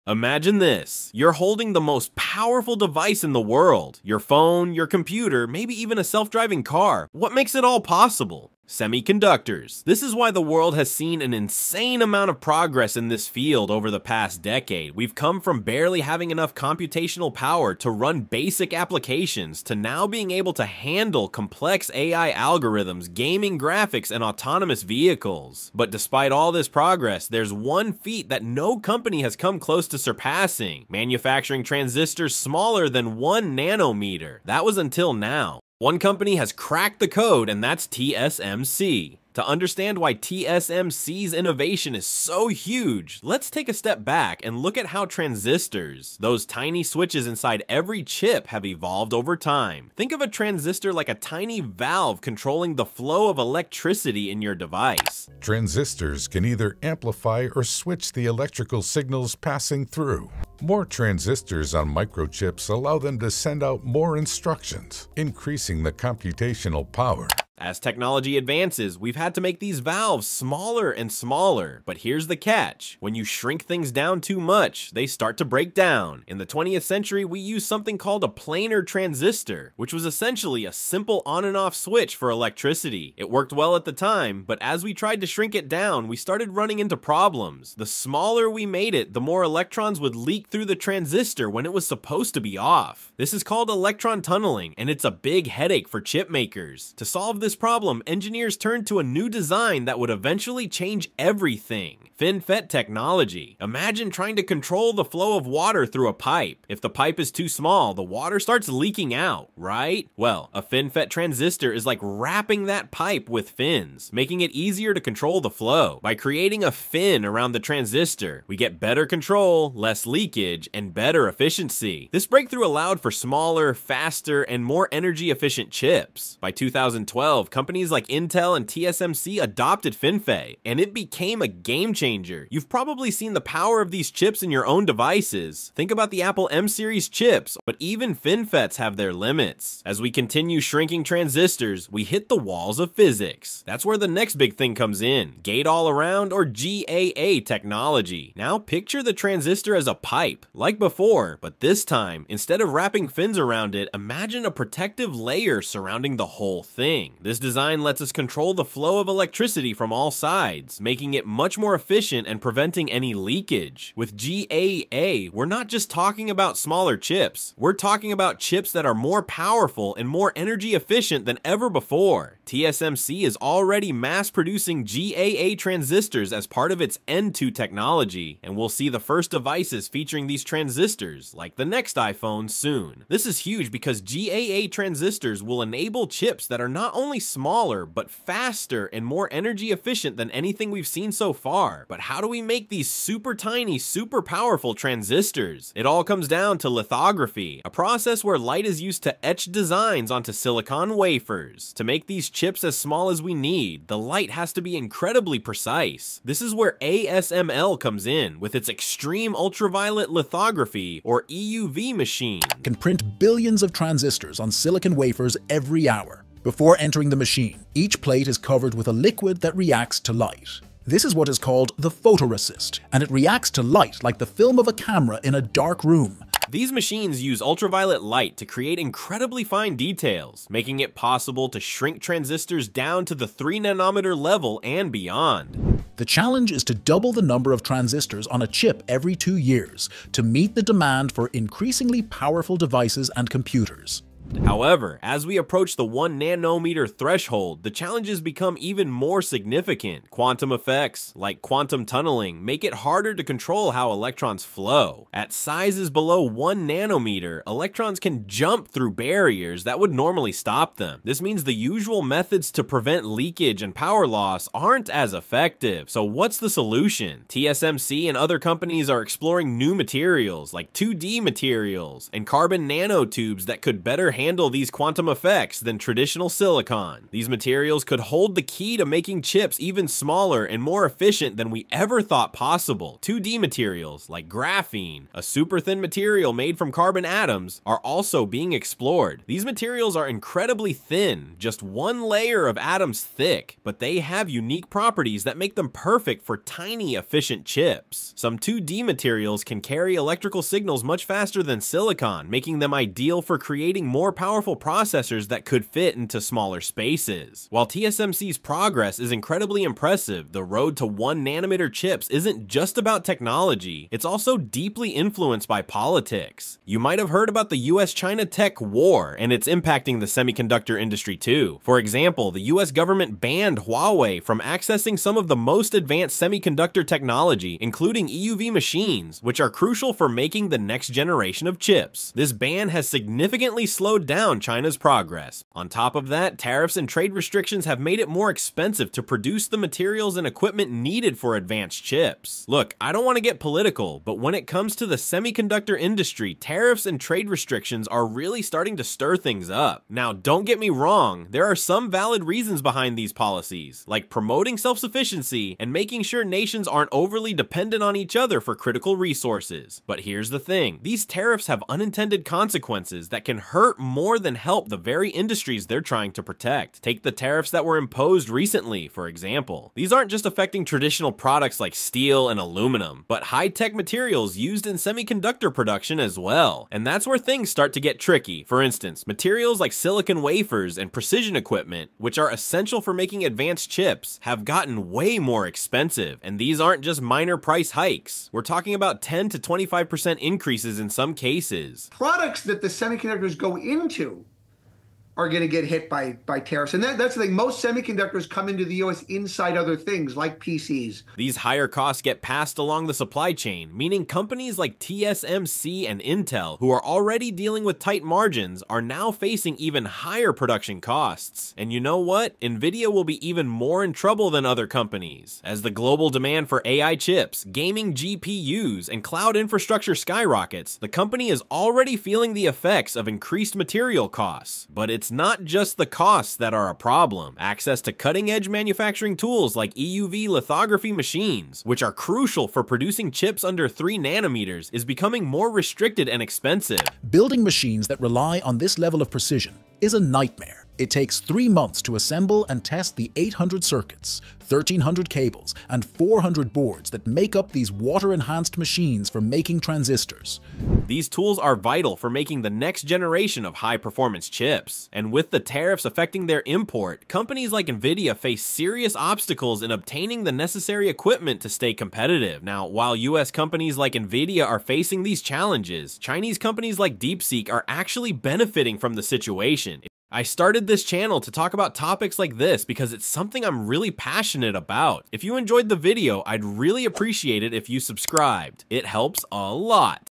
Huge 1nm Breakthrough Breakthrough - The Future of AI Chips
TSMC just cracked the 1-nanometer barrier—a milestone that seemed impossible just a few years ago. In this video, we break down why this innovation is groundbreaking for AI, smartphones, gaming, and even self-driving cars. We dive deep into the evolution of transistors, the shift from FinFET to GAA technology, and how companies like TSMC, Intel, and NVIDIA are navigating a politically charged semiconductor landscape. From EUV lithography to 2D materials like graphene, we cover it all.

Whether you're a tech enthusiast or just curious about how your devices keep getting faster and smaller, this is a story you don't want to miss.

Chapters:
0:00 - Intro: The Most Powerful Tech You Hold
0:43 - Why Semiconductors Matter More Than Ever
1:25 - How Transistors Work (And Why Size Matters)
2:10 - From Planar to FinFET: The Revolution of the 2010s
3:00 - The Rise of GAA: The Next-Gen Transistor Design
4:15 - TSMC's N2 Breakthrough and What It Means for You
5:00 - The Power of EUV Lithography and ASML's Role
6:00 - The Challenges Below 1nm: Quantum Tunneling & New Materials
6:45 - Graphene, 2D Materials & The Silicon Replacement Race
7:30 - Politics & Tariffs: How the Tech War is Changing the Game
8:10 - Who Wins and Who Loses in the New Chip Era?
8:30 - Outro: What’s Next for Chips and AI?